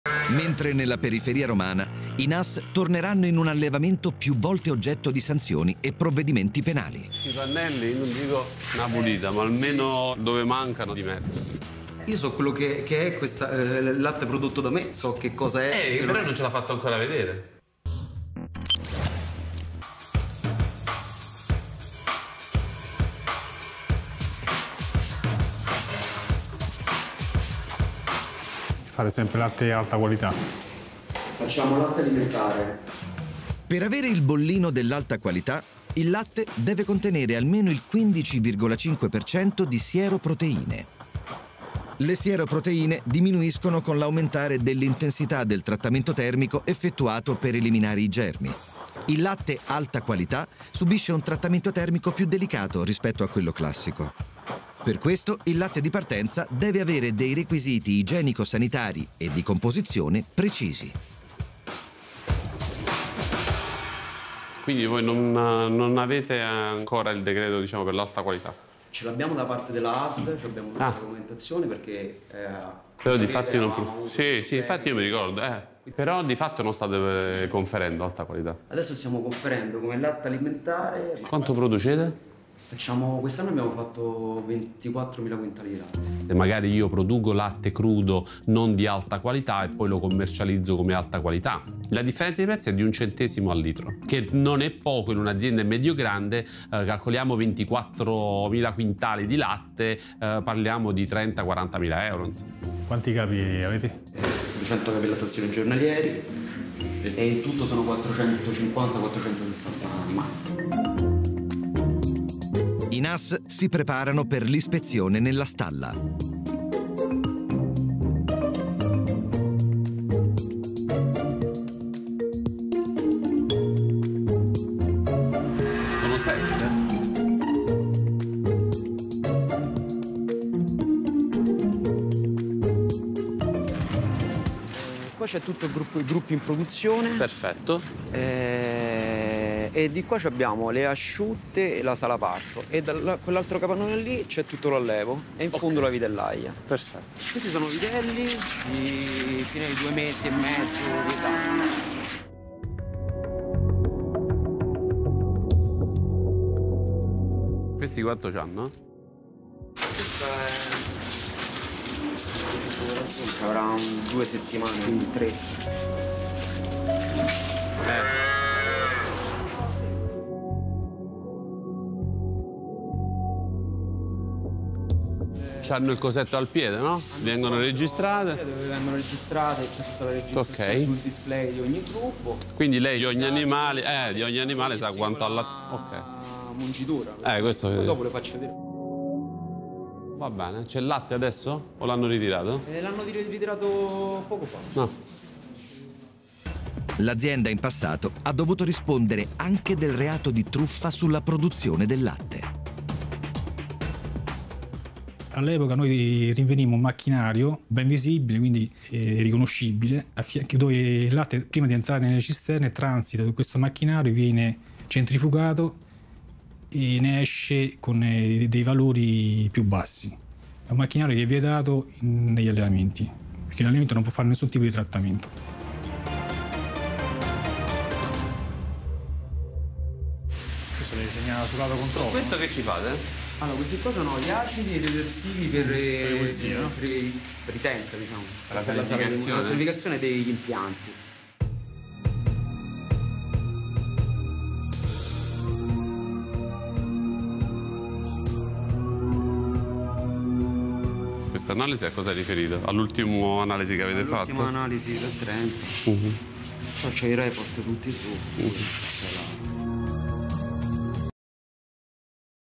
I NAS sono nella periferia di Roma per controllare un allevamento che ha già subito diversi richiami
Tutti i giorni i N.A.S. difendono la salute dei cittadini! Seguiamo da vicino le loro indagini, saranno autorizzati e sanificati i luoghi che controlleranno?